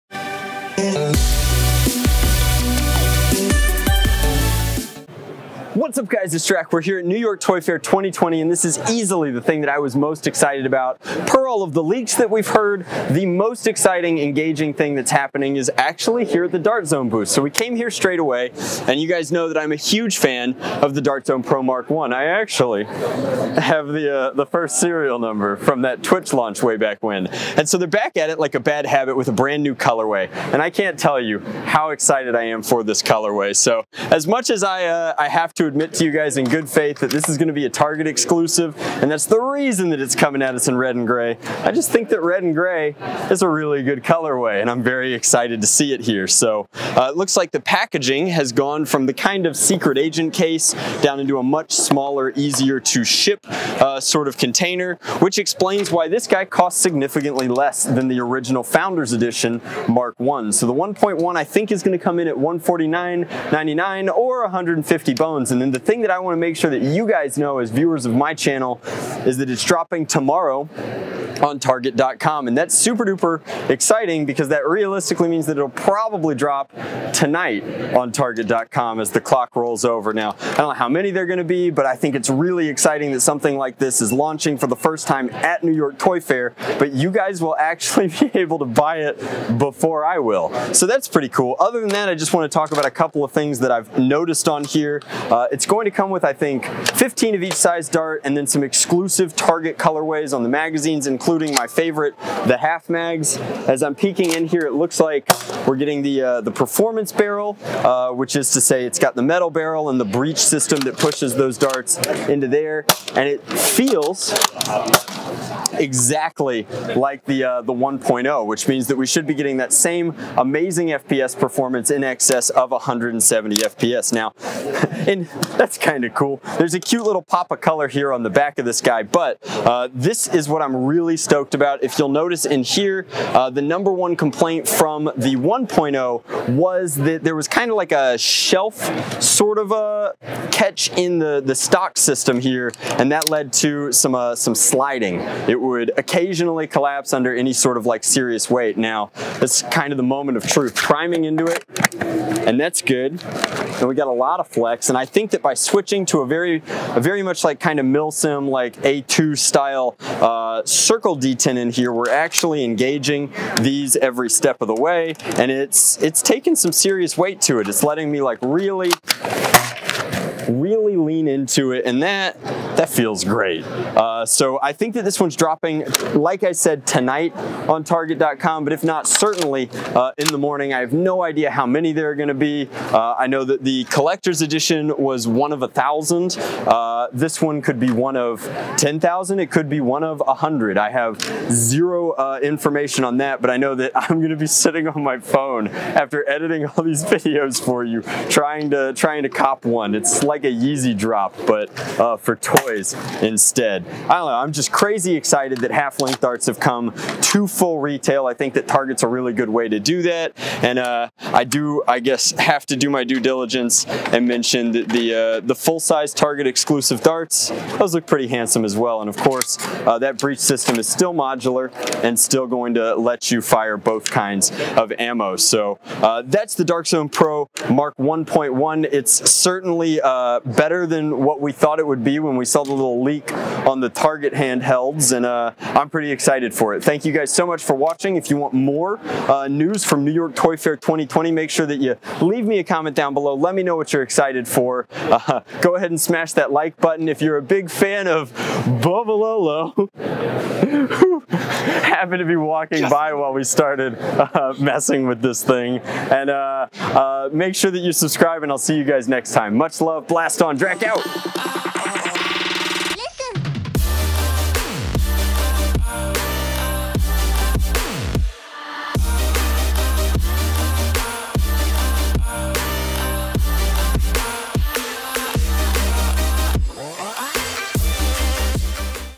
New York Toy Fair 2020: The Dart Zone Pro Mk1.1!
A new run of the best blaster of 2019.

My fanmail address is:
Drac Thalassa